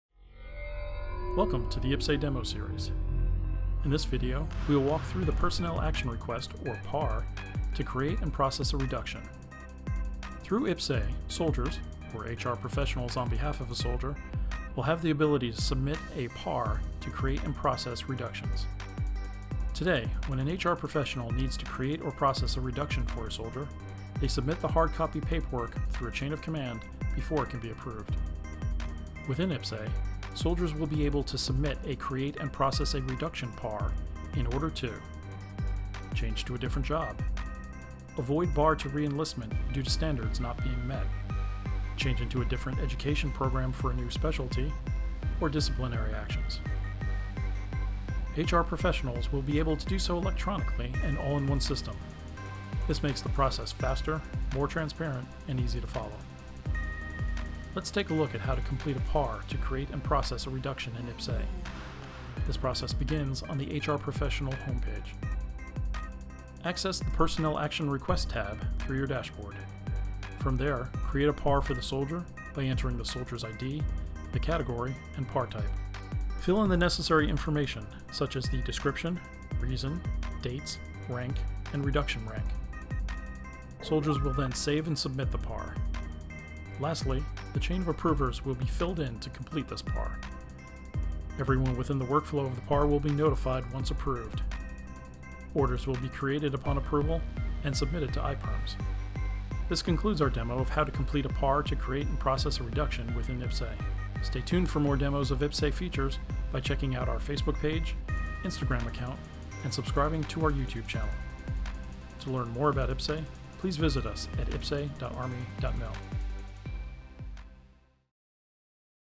IPPS-A Demo Series: PAR Create and Process a Reduction
This is the eighth installment of the IPPS-A Demo Series. In this demo,  we walk you through how Soldiers and HR Professionals will submit a PAR to Create and Process Reductions in IPPS-A.